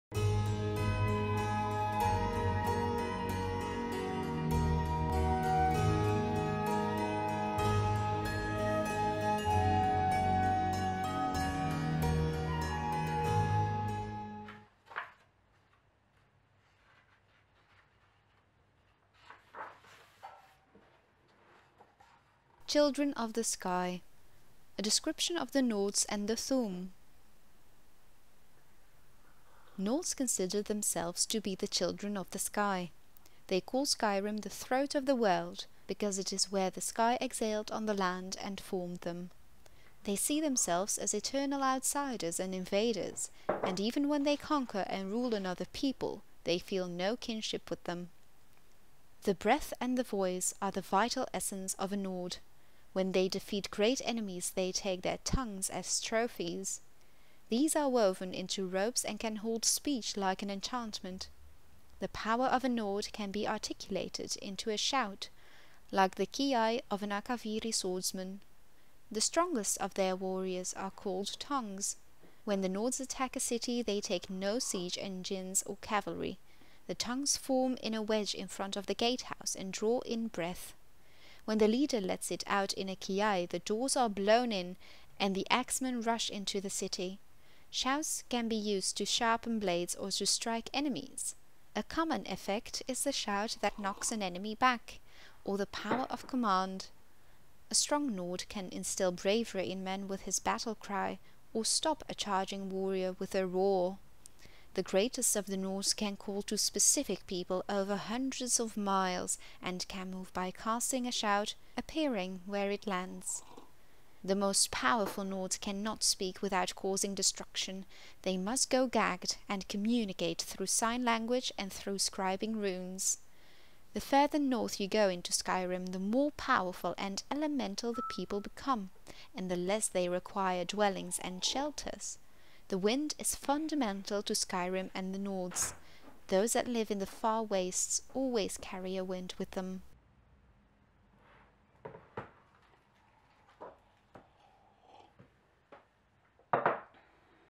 Skyrim audiobook - Children of the Sky
Explore the wondrous world of Skyrim with a distinct cup of tea and snack, while reading along the beautiful Skyrim Library trilogy containing one book for all History related writings, one for all Man, Mer and Beasts and one for all things Arcane.
We're continuing to read in Book Two: Man, Mer and Beasts. Its first chapter was Wolf-Heart's essay to redeem Nords in Tamriel, whereas this next chapter goes more in depth to describe the Nords, or Children of the Sky, and their Thu'um or shout.
The tea poured into this video is Kukicha - beautifully warm and amber in both colour as taste, this Japanese smoked tea has a nutty flavour accompanied by vanilla tones and a slightly hidden grassiness to cover your entire palate.